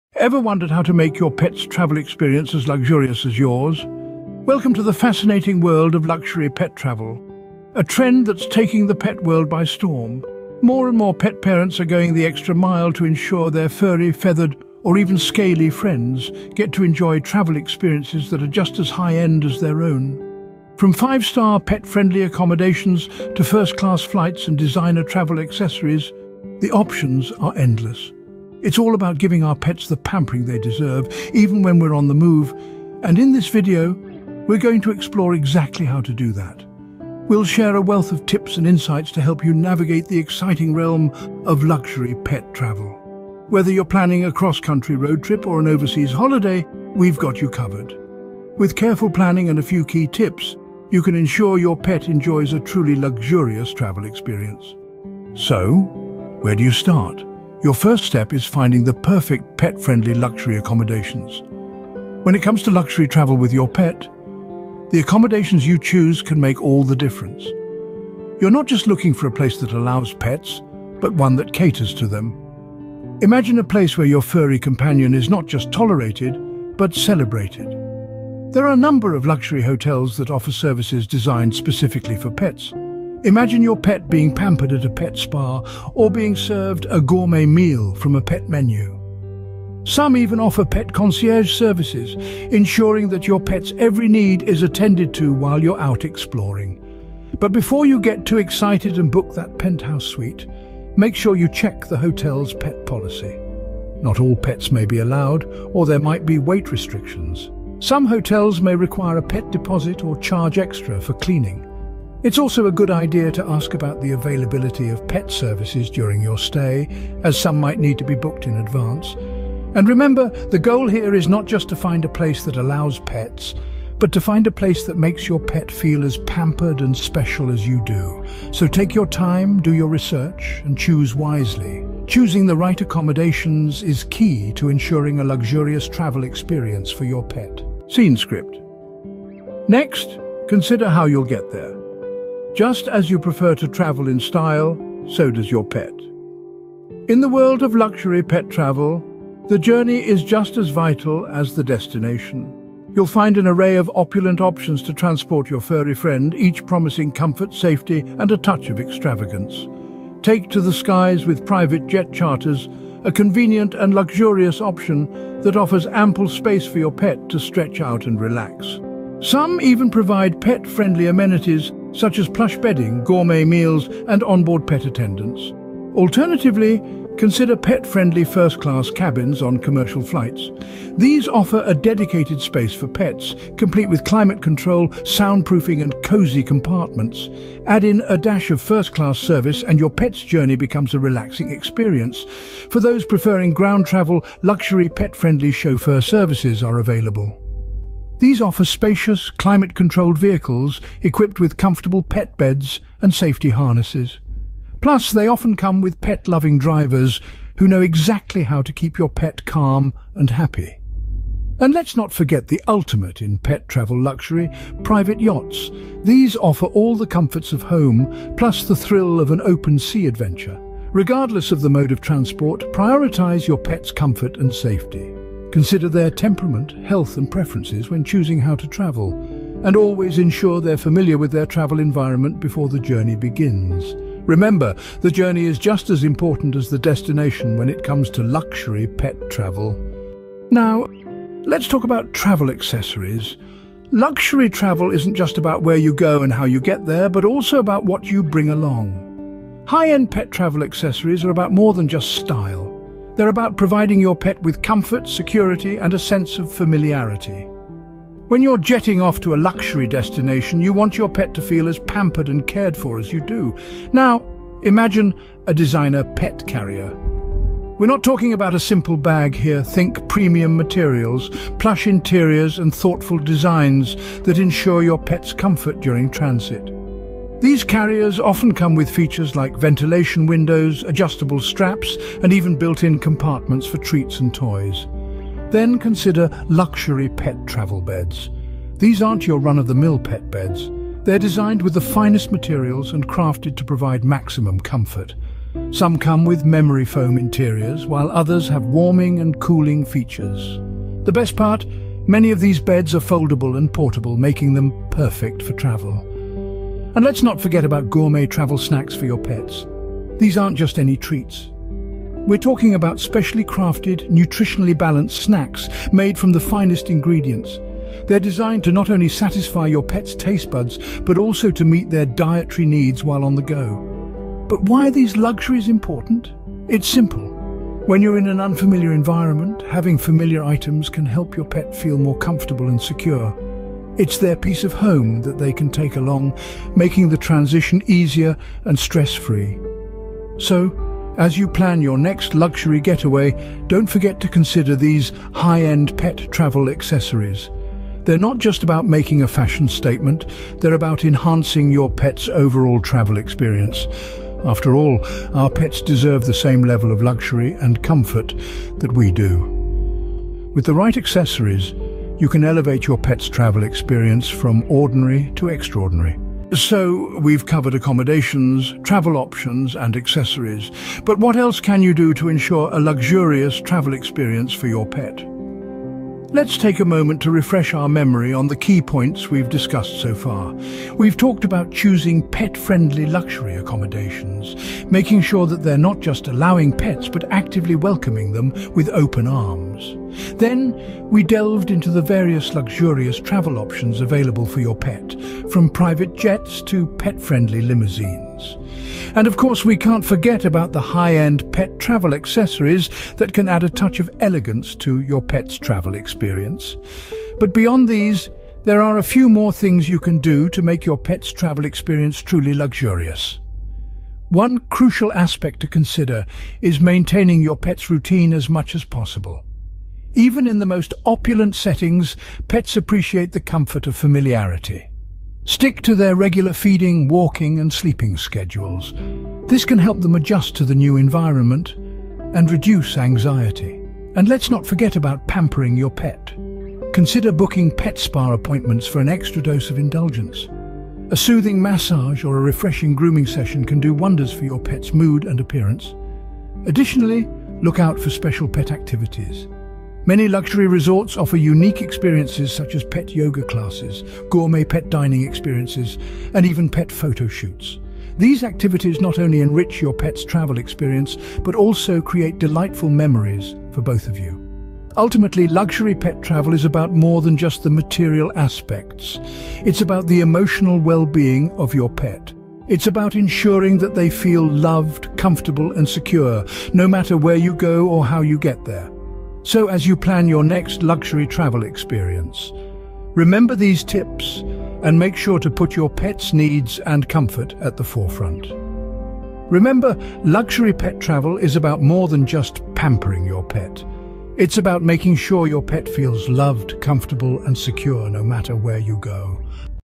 Everything You Need to Know About Luxury For Your Pets | Travel Hacks 2024
In this video, we are diving into the wonderful world of luxury pet travel. Whether you're planning a getaway with your furry friend or simply seeking inspiration, we've got you covered with top-notch advice and tips to ensure unforgettable experiences for your pets on the go.

Join us as we explore the best practices for pampering your pets during travels. From luxury pet-friendly accommodations to exclusive pet-friendly activities, we'll guide you through the process of creating memorable moments for your beloved companions.

Discover the hottest destinations that cater to pets, as we showcase luxurious resorts and hotels specifically designed to provide comfort and enjoyment for your four-legged pals. Our expert recommendations will help you find the perfect accommodations that go above and beyond, ensuring an indulgent stay for both you and your pet.

Learn essential tips on transportation options that prioritize your pet's comfort and safety. From pet-friendly airlines to luxury pet travel services, we'll cover it all. You'll gain insights into pet travel essentials, such as carriers, packing tips, and guidelines for a stress-free journey for your furry friends.

As pet lovers ourselves, we understand the importance of holistic well-being for our furry companions. That's why we'll also delve into the world of luxury pet spas, gourmet pet food, and personalized pet services that cater to your pet's every need. Prepare to be amazed by the array of offerings that will make your pet feel like a true VIP.

We can't wait to embark on this luxurious pet travel adventure with you! Let's ensure our pets experience the ultimate in luxury during our travels.

OUTLINE:
00:00:00
Introduction to Luxury Pet Travel
00:00:54
Choosing the Right Pet-Friendly Luxury Accommodations
00:02:17
Luxury Travel Options for Pets
00:04:00
High-End Pet Travel Accessories
00:06:06
Wrapping Up Luxury Pet Travel Tips